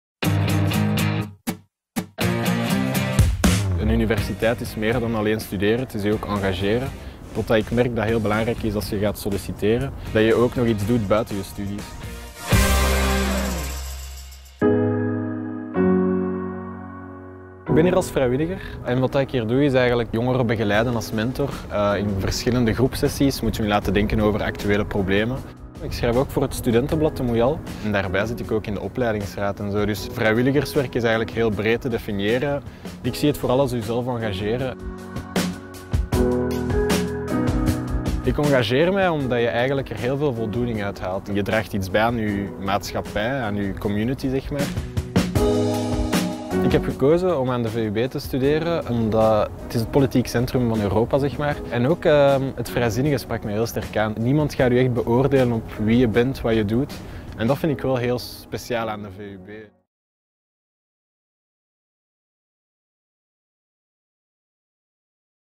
VUB-vrijwilliger Jade Pieters
"Wat ik merk dat heel belangrijk is als je gaat solliciteren, is dat je ook nog iets doet buiten je studies (...) Ik zie vrijwilligerswerk vooral als jezelf engageren."
De wereld heeft Jade nodig. En jou. Zie dewereldheeftjenodig.be.